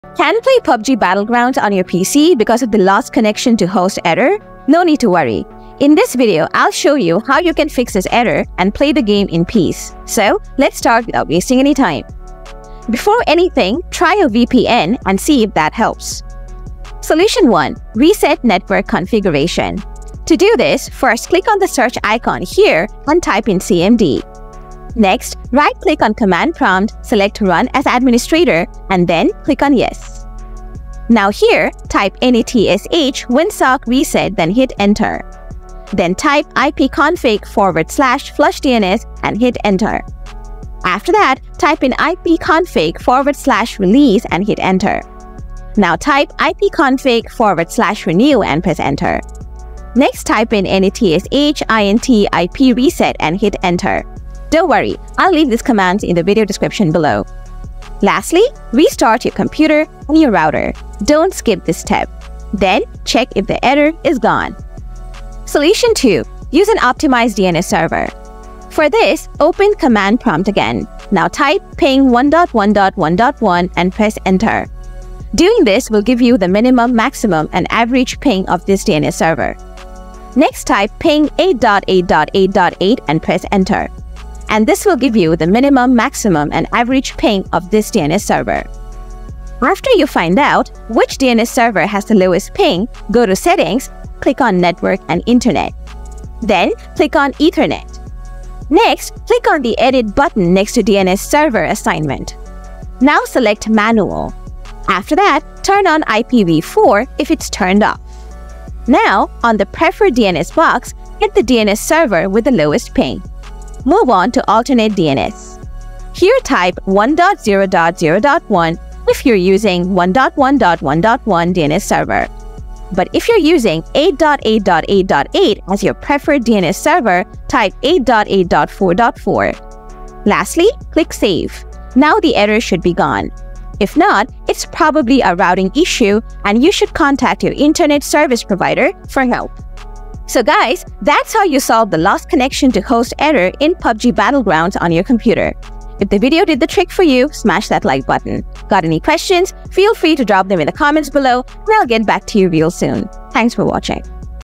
How to Fix "Lost Connection to Host" Error on PC | Lost Connection to Host Issues on PC
Getting a "Lost Connection to Host" error while gaming or using applications on your PC can be frustrating, especially when you're in the middle of something important. This error often occurs due to network instability, firewall interference, or server issues. In this guide, we'll cover simple and effective solutions to help you fix lost connection to host errors and ensure smoother performance on your PC. Follow our step-by-step instructions to troubleshoot and resolve this issue.

Here are the commands mentioned in the video:
1. netsh winsock reset
2. ipconfig /flushdns
3. ipconfig /release
4. ipconfig /renew
5. netsh int ip reset